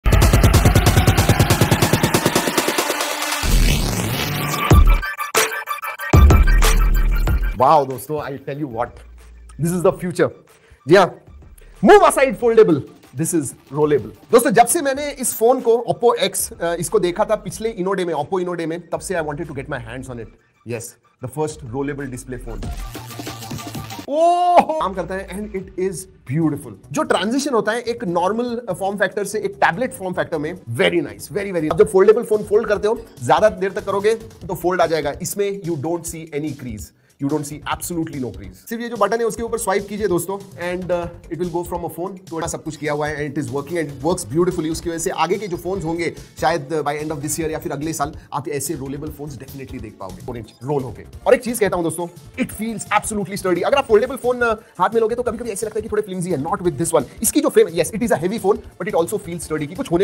OPPO X 2021 Rollable Phone Hands On & First Impressions | 125W Charger 😮 ⚡ This Is The Future
Doston aaj ke video me hum ek bahot hi interesting device ke baare me baat karne wale hain. Ye ho sakta hain humare smartphones ka future, to aap ye video ant tak jaroor dekhiye aur video ko like aur share karna na bhoole.